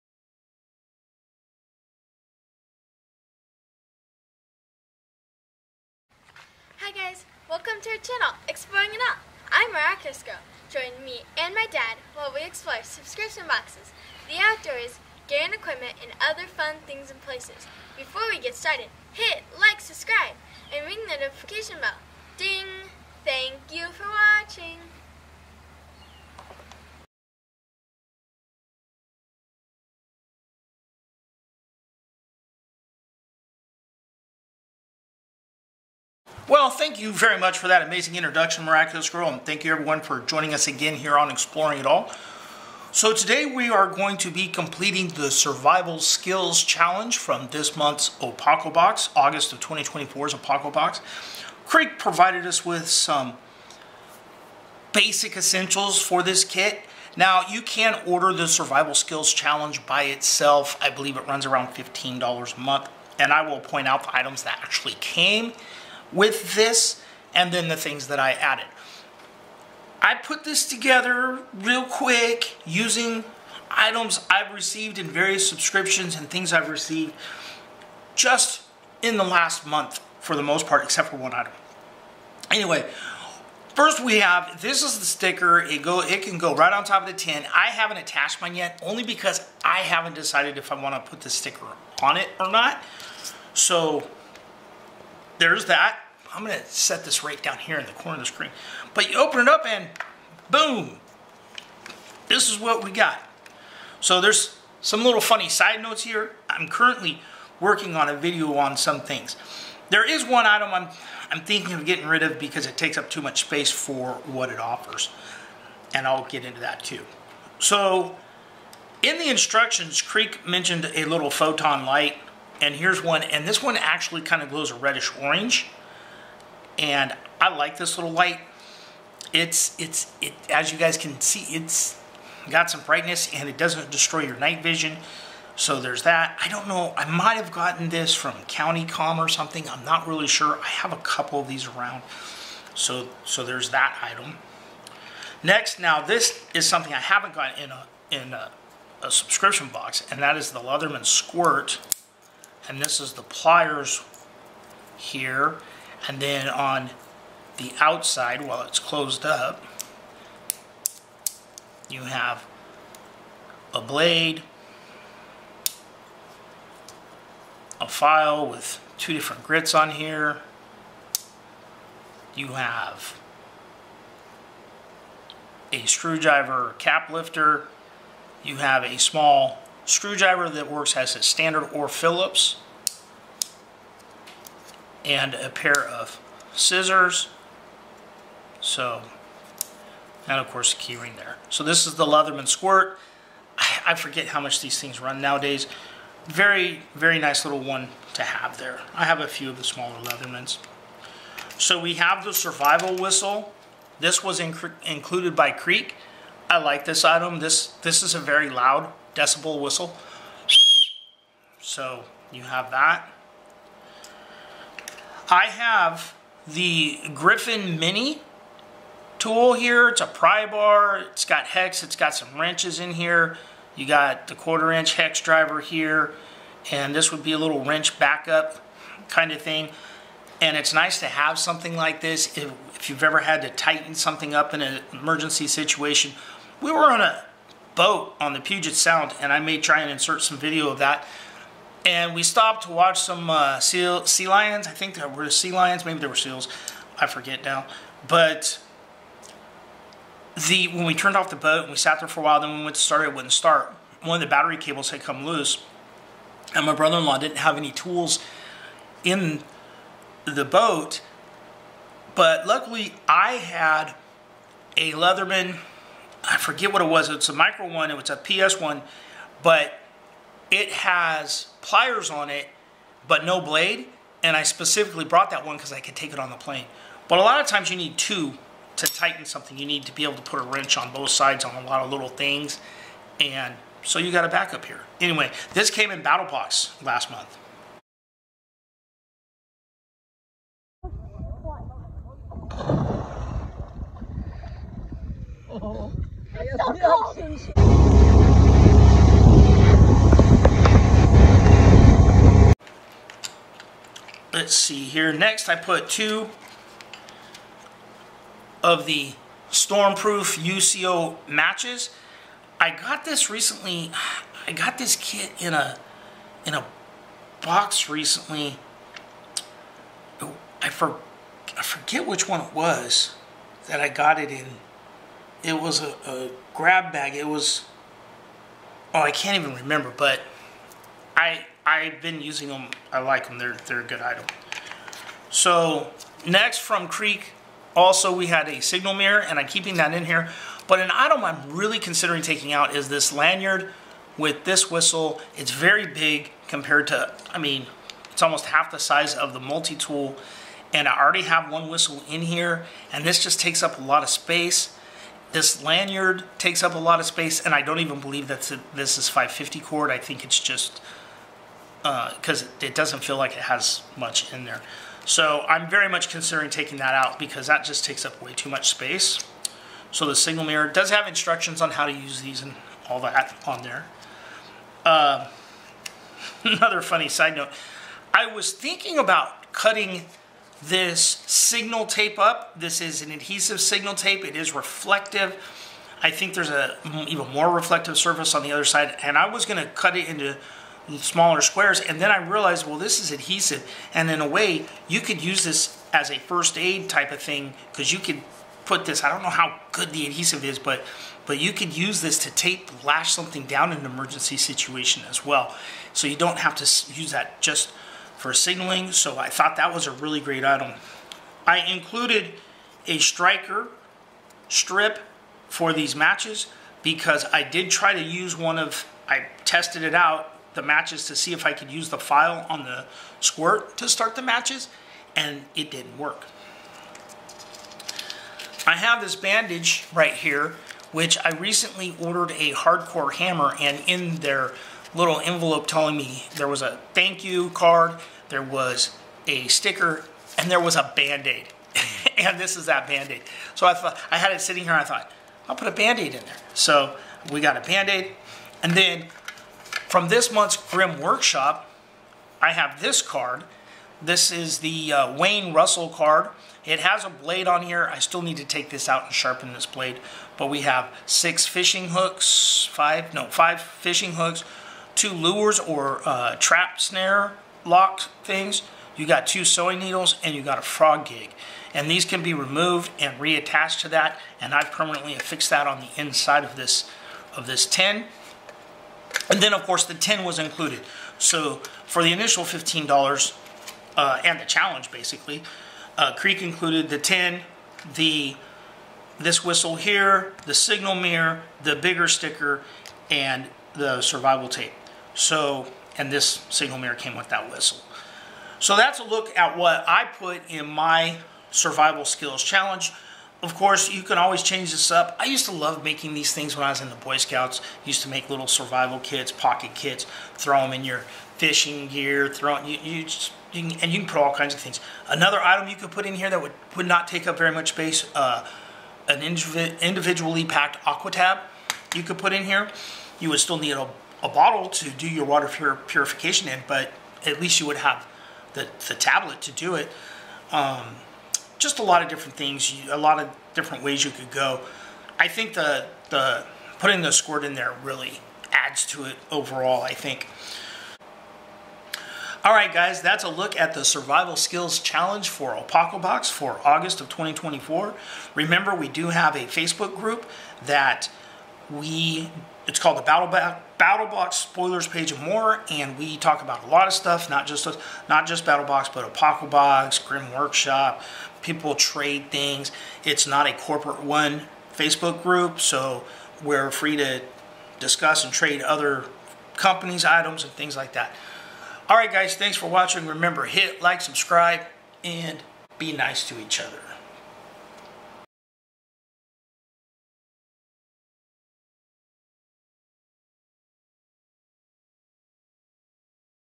Apocabox Survival Skills Challenge August 2024
The August Survival Skills challenge reminded me of my old boy scout days! I loved making survival kits! Lets look what we threw together here!